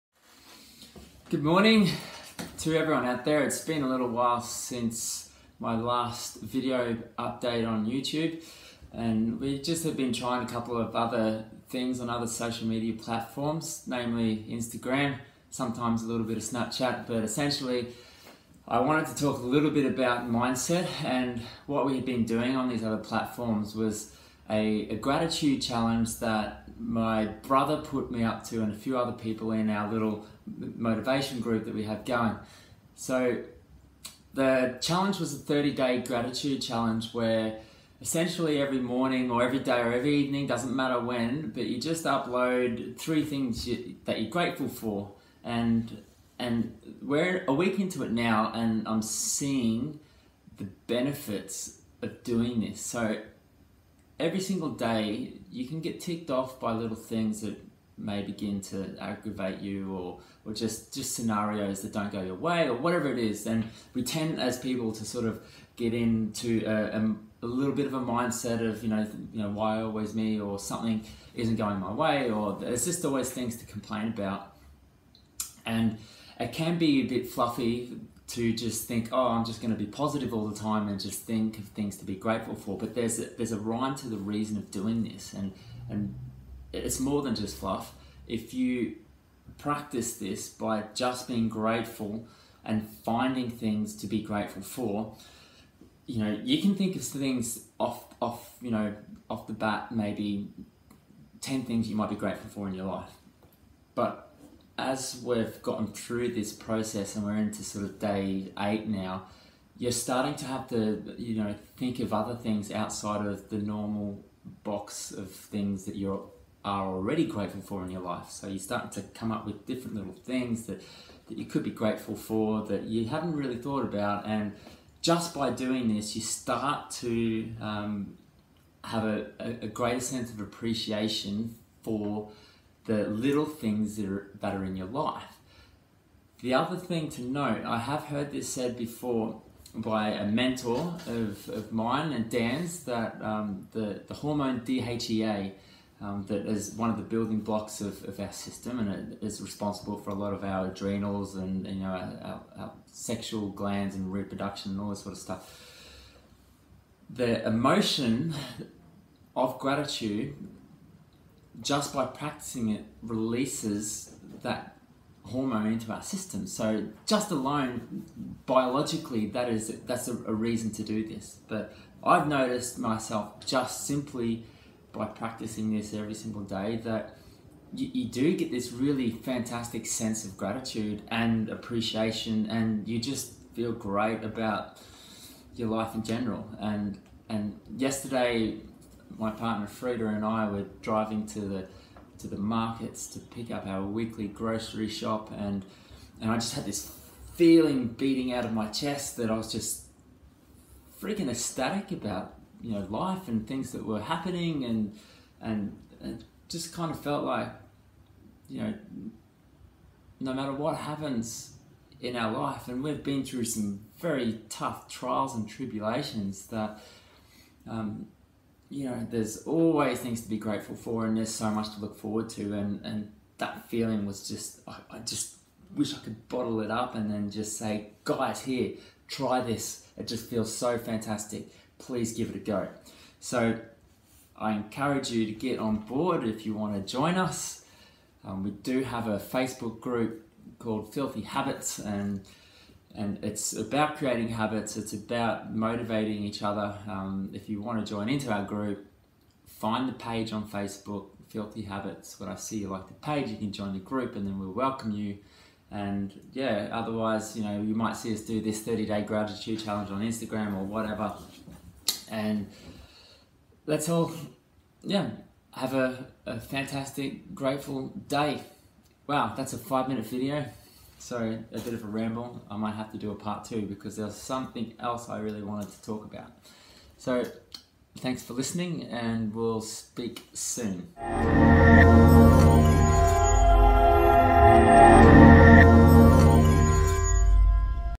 SSE Episode 15: [Benefits of the Gratitude Challenge] 💪
My brother set me to a 30 Day Gratitude challenge and only 1 week in I’m already seeing the transformation that occurs... honestly I encourage everyone to give this a try. It takes 2 minutes of your day. If you don’t have 2 minutes to be grateful well then I guess you are a miserable sod.